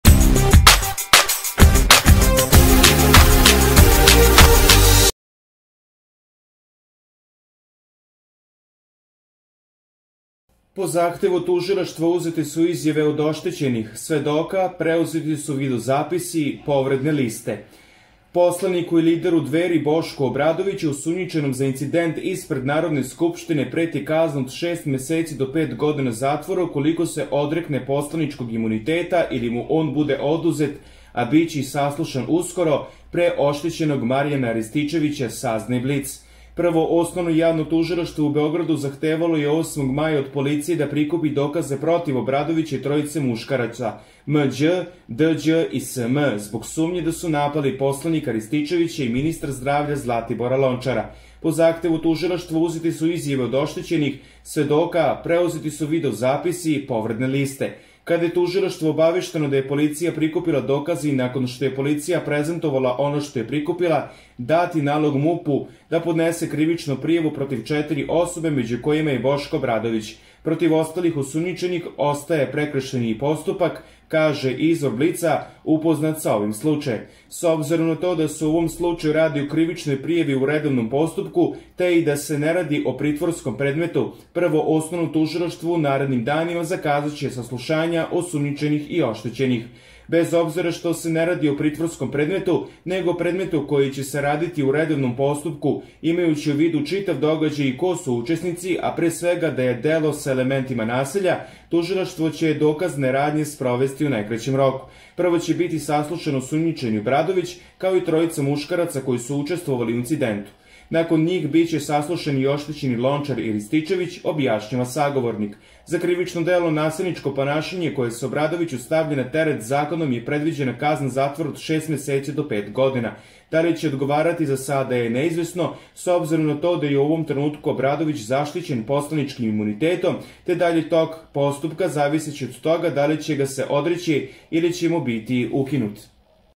HITNO: Boško Obradović ide u zatvor! PRIKUPLJAJU SE NEOBORIVI DOKAZI
Po zahtevu tužilaštva uzete su izjave od oštećenih, svedoka, preuzeti su video zapisi, povredne liste...